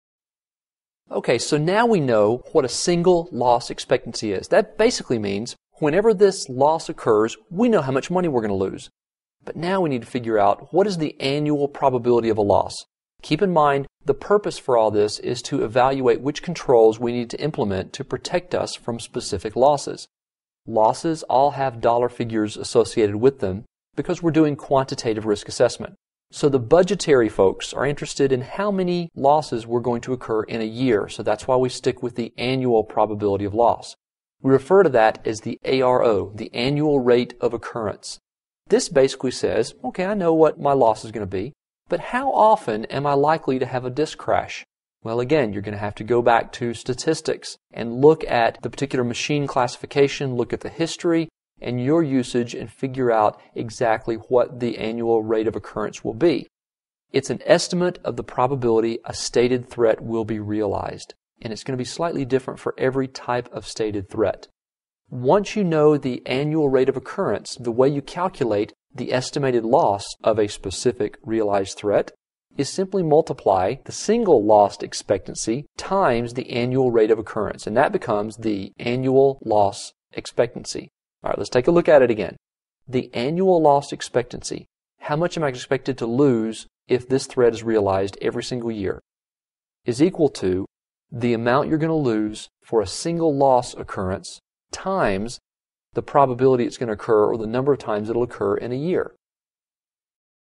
06 Mastering Single Loss Expectancy & Annual Rate of Occurrence for Better Budgeting
Understanding the concept of Single Loss Expectancy (SLE) is crucial for our quantitative risk assessment. It allows us to determine how much money we stand to lose when a specific loss occurs. However, it's equally important to calculate the Annual Rate of Occurrence (ARO), which helps us estimate how often these losses might happen within a year.

By analyzing historical data and machine classifications, we can accurately assess the ARO for various threats. This video will guide us through calculating Annual Loss Expectancy (ALE) by multiplying SLE with ARO, providing insights into potential financial impacts from realized threats.